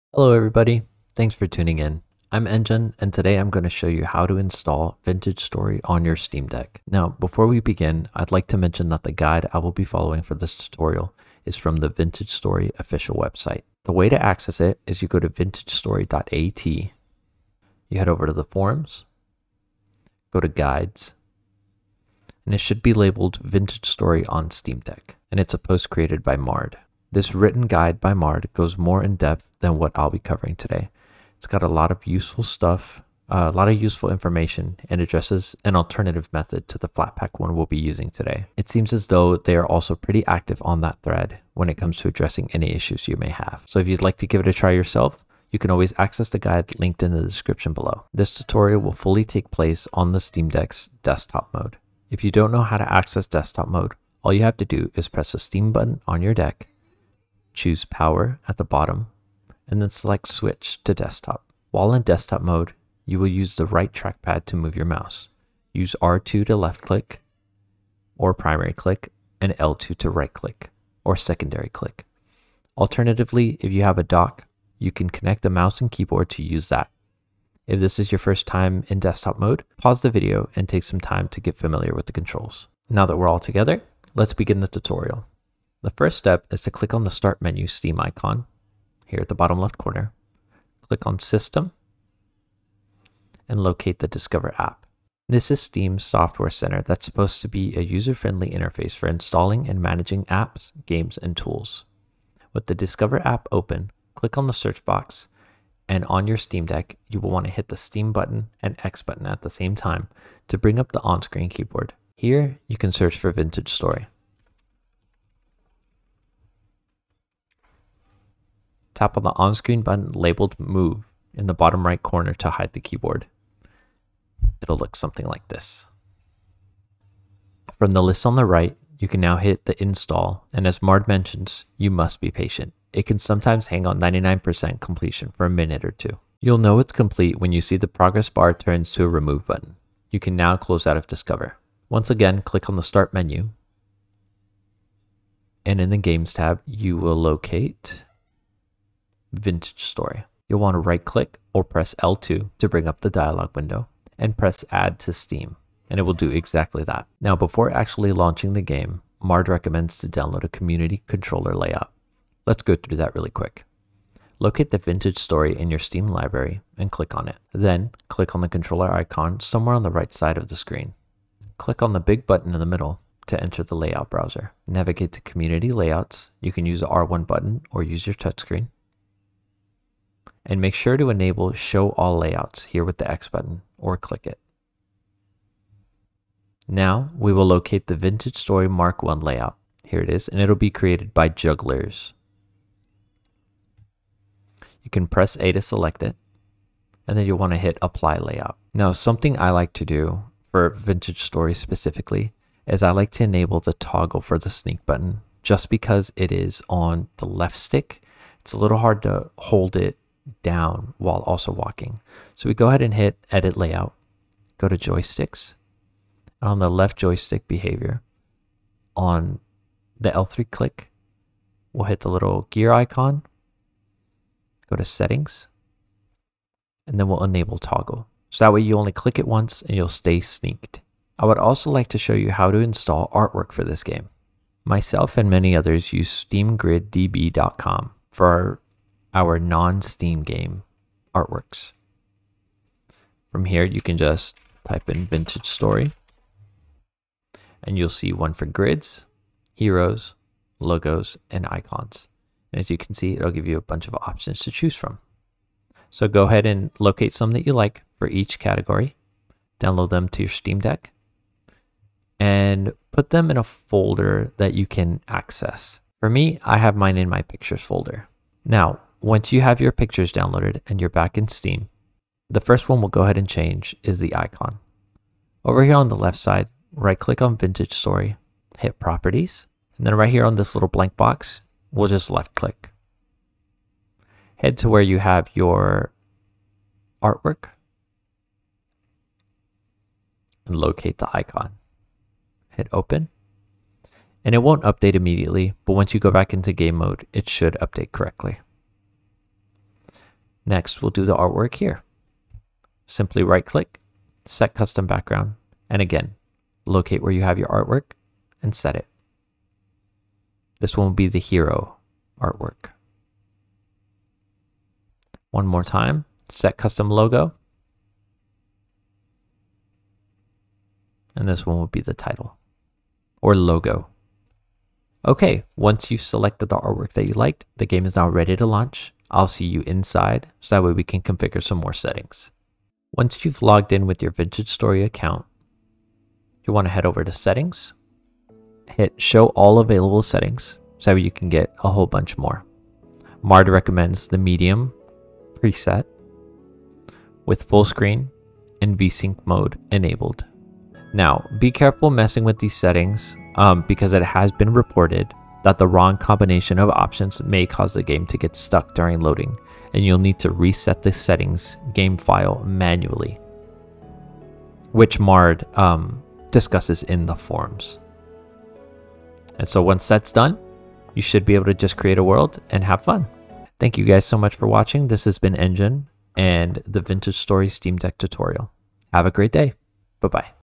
NEW How to Install Vintage Story on Steam Deck
Hello everybody! Here is a tutorial for how to install Vintage Story on your very own steam deck!

This new one is recorded with a better mic than the old one was. I hope this one is clearer! leave your questions in the comments!